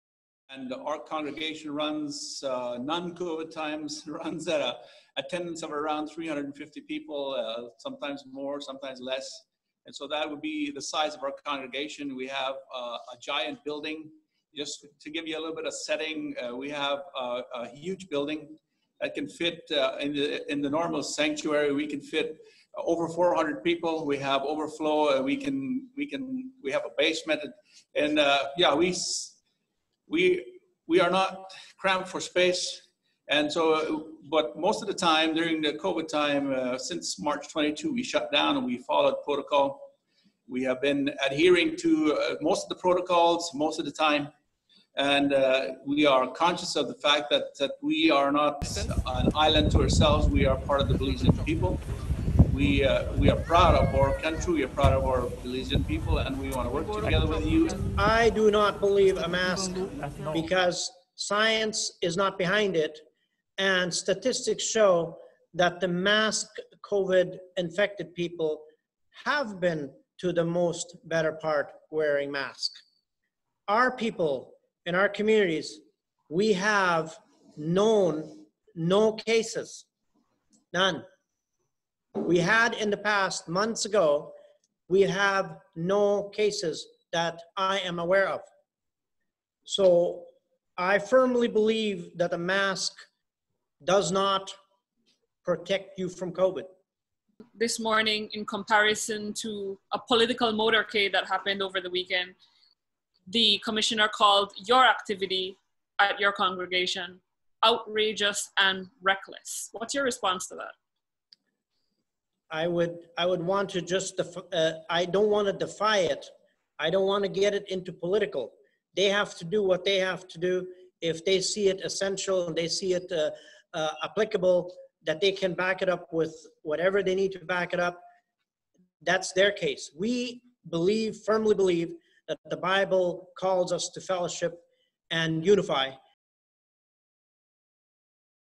Blue Creek Mayor Objects to Mask Wearing
In standing in solidarity with the local church community, Evangelical Pastor John Krahn also says that his church has a capacity of over four hundred persons but since March, due to the COVID-19 regulations, they have been following the protocols but are asking government to reconsider its position on reopening of churches.  Blue Creek Mayor Abram Rempel, in speaking individually, has taken a strong position against the use of face masks.